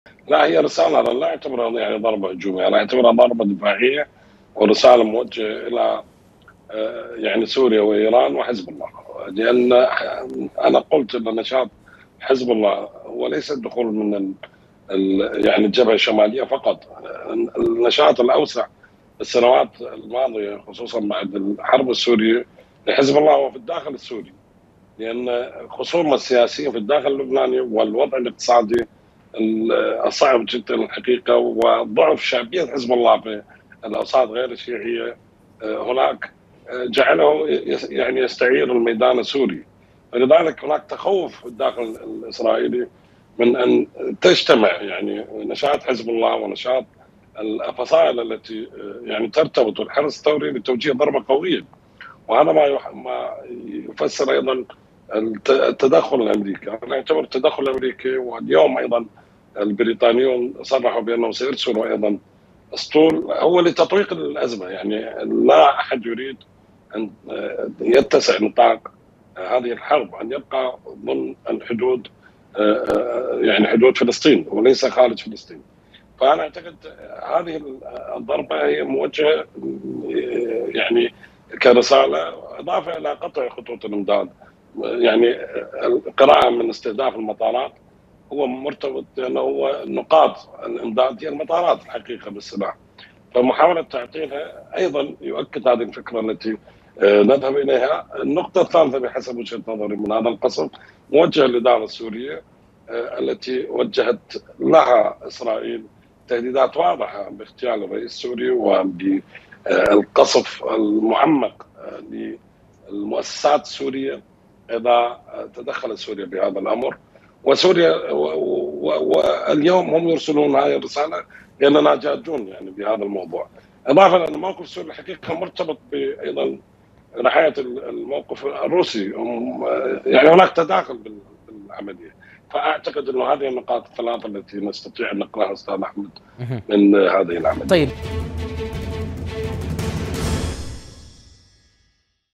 الياسري: الضربات الاسرائيلية على مطارات سوريا رسالة واضحة المعالم
موقع قناة التغيير الفضائية  الالكتروني